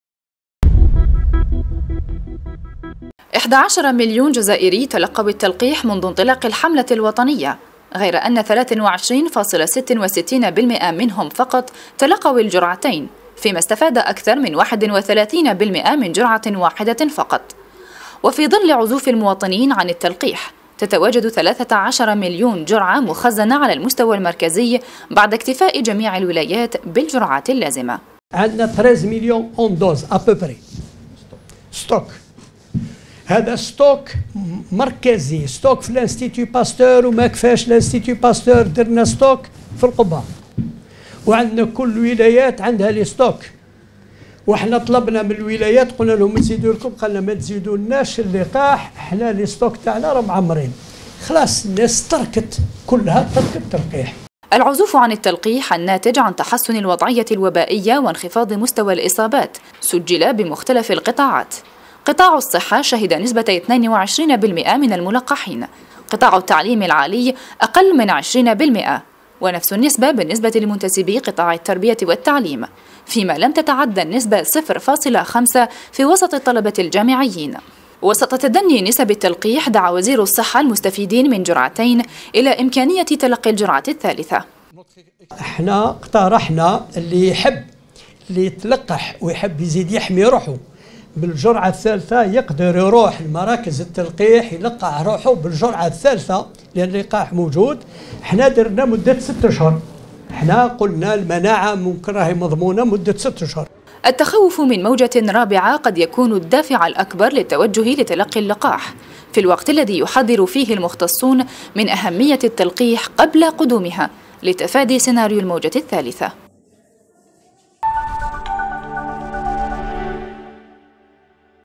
بن بوزيد: 23 % من الملقحين تلقوا الجرعتين ويمكنهم تلقي الجرعة الثالثة بعد 6 أشهر
الجزائر (وأج)- كشف وزير الصحة عبد الرحمان بن بوزيد، عن تراجع الإقبال على عملية التلقيح على المستوى الوطني بعد الاستقرار في الحالة الوبائية، مبرزا خلال اشرافه على احياء اليوم الوطني للمتبرعين بالدم، أن 23 % من الملقحين تلقوا الجرعتين، بينما اكد إمكانية  تلقي هؤلاء للجرعة الثالثة بعد مرور 6 أشهر.